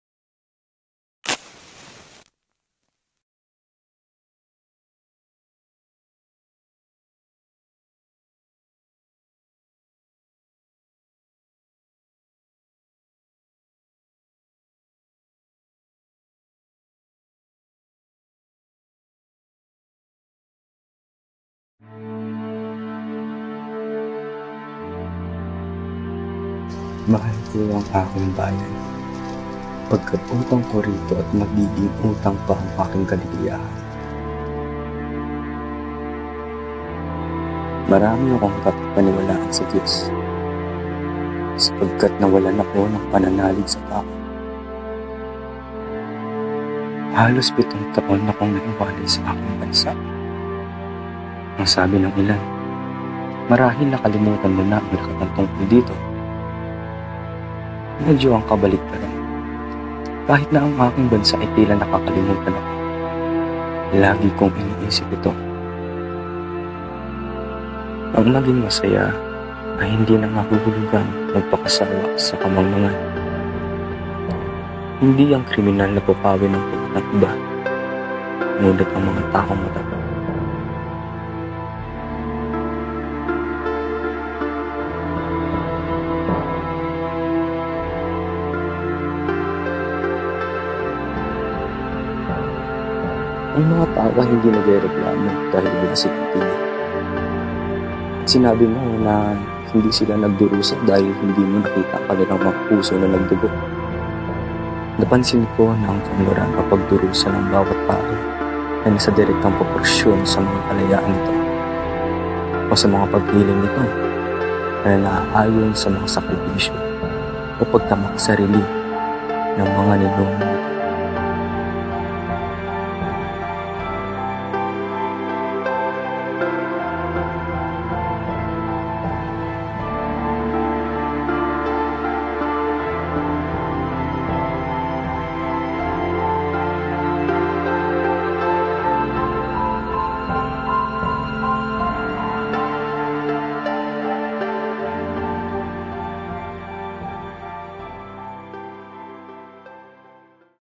Mahal ko ang aking Bayan: A Monologue of Crisostomo Ibarra
This video is dedicated to our subject "The Life and Works of Rizal" a video created by Angelo Caña of BS Psychology 2. All used words/phrases are from its reference "Noli Me Tangere."